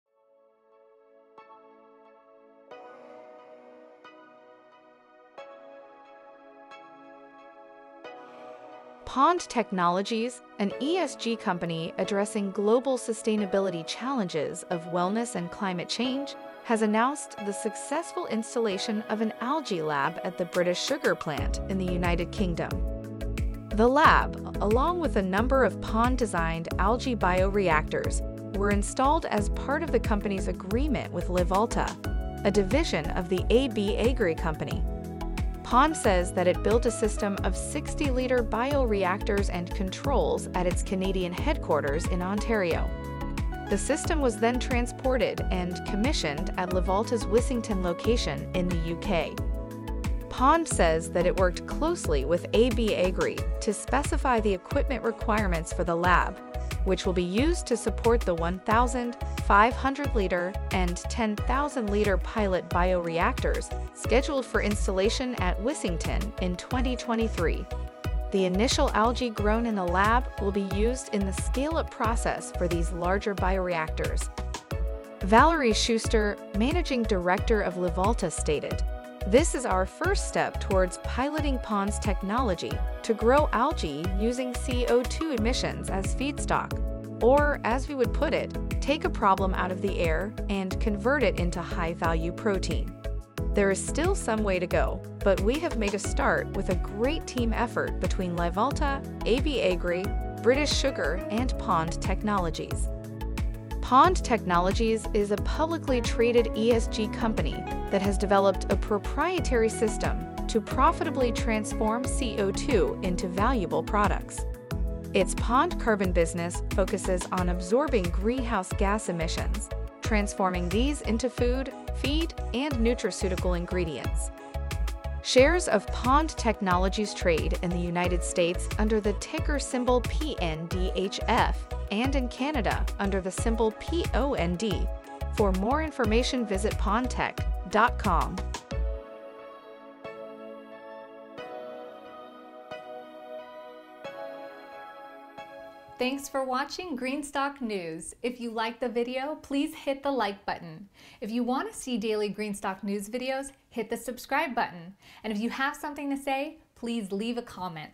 Pond Technologies ($PNDHF) Installs Proprietary CO2 Reduction Algae System at Livalta Sugar Plant
Pond Technologies (OTCMKTS: PNDHF),an ESG company addressing global sustainability challenges of wellness and climate change, has announced the successful installation of an algae lab at the British Sugar plant in the United Kingdom.

The lab, along with a number of Pond-designed algae bioreactors, were installed as part of the company's agreement with Livalta, a division of the AB Agri company. Pond says that it built a system of 60-liter bioreactors and controls at its Canadian headquarters in Ontario. The system was then transported and commissioned at Livalta’s Wissington location in the UK.

Pond says that it worked closely with AB Agri to specify the equipment requirements for the lab, which will be used to support the 1,500-liter and 10,000-liter pilot bioreactors scheduled for installation at Wissington in 2023. The initial algae grown in the lab will be used in the scale-up process for these larger bioreactors.

Valerie Schuster, Managing Director of Livalta stated: "This is our first step towards piloting Pond's technology to grow algae using CO2 emissions as feedstock - or, as we would put it, take a problem (CO2 emissions) out of the air and convert it into high value protein. There is still some way to go, but we have made a start with a great team effort between Livalta, AB Agri, British Sugar and Pond Technologies."

Pond Technologies is a publicly traded ESG company that has developed a proprietary system to profitably transform CO2 into valuable products. Its Pond Carbon business focuses on absorbing greenhouse gas emissions, transforming these into food, feed, and nutraceutical ingredients.

Shares of Pond Technologies trade in the United States under the ticker symbol PNDHF and in Canada under the symbol POND.